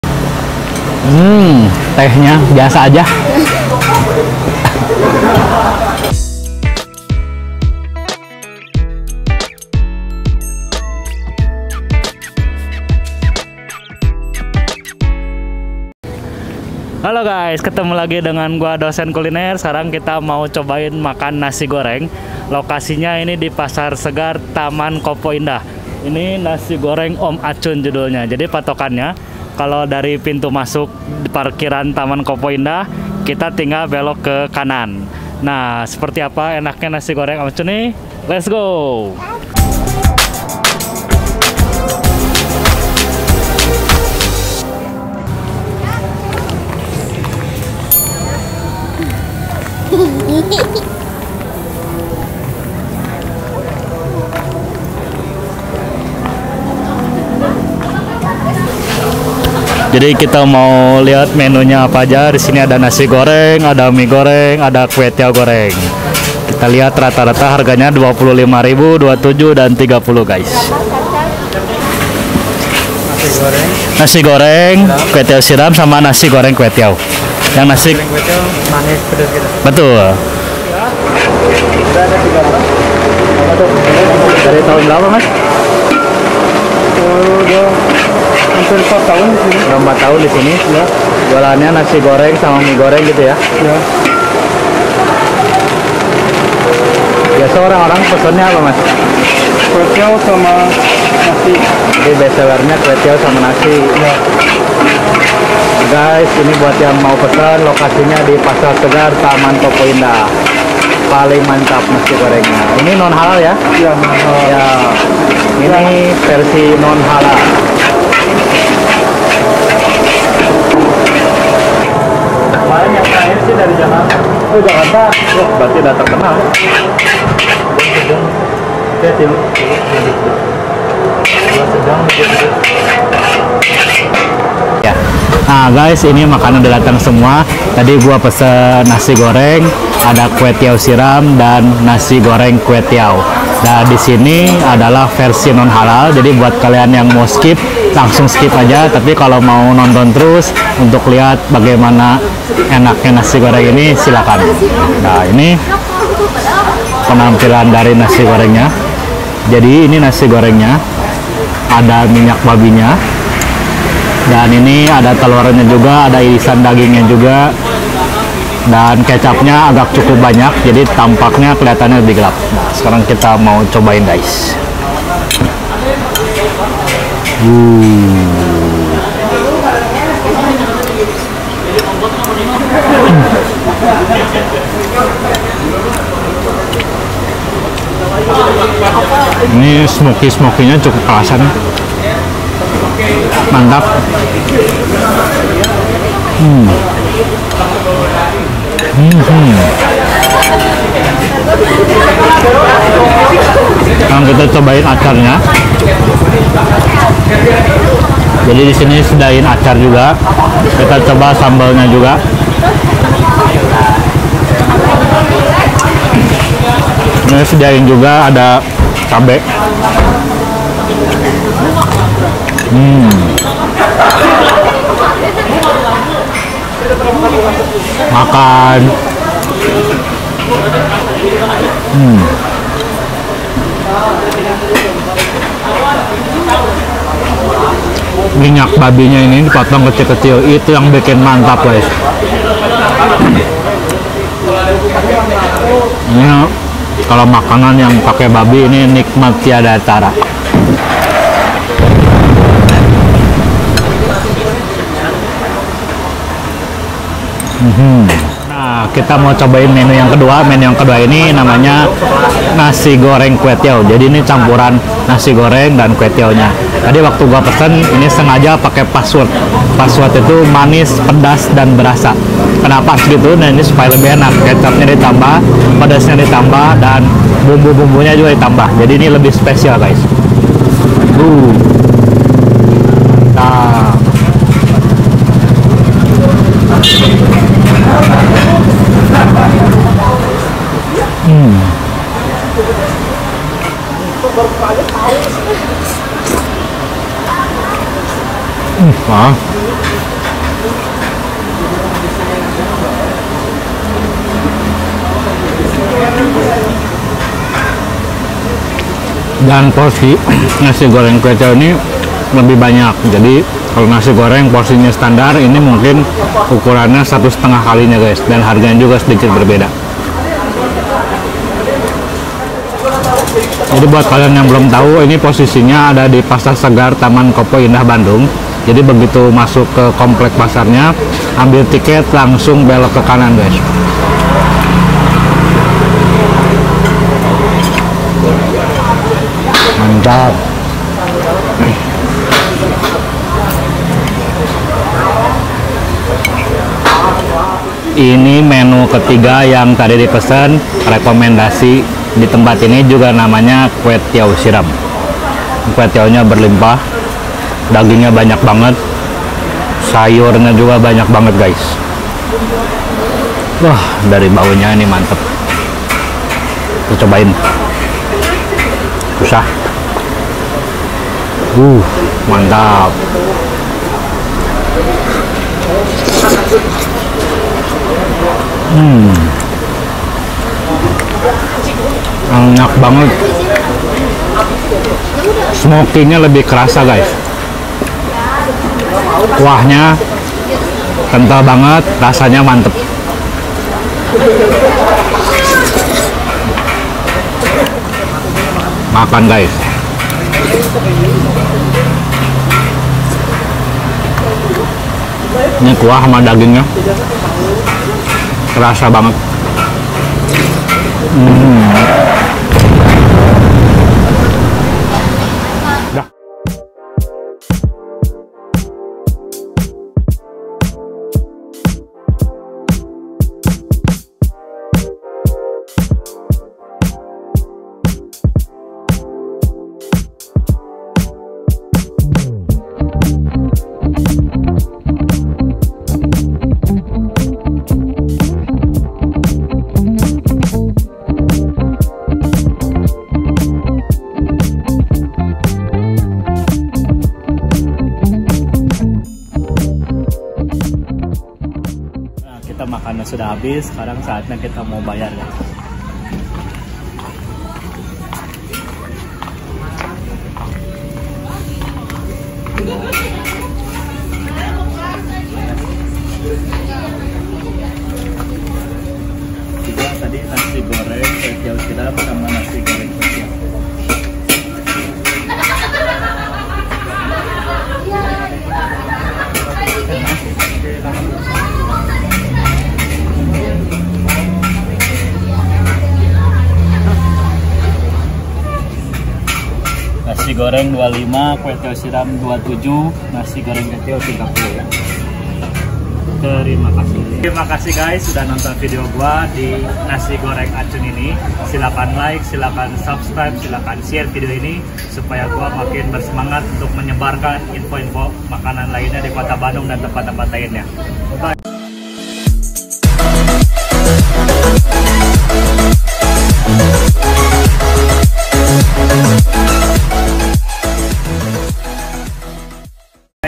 YANG PAKE BABI PASTI MANTAP! NASI GORENG ENAK NIH...
Thank you sudah nonton video ini, semoga kalian suka!

NASI GORENG OM ACUN
Kompleks Pasar Segar
TAMAN KOPO INDAH II
BANDUNG
Cash Only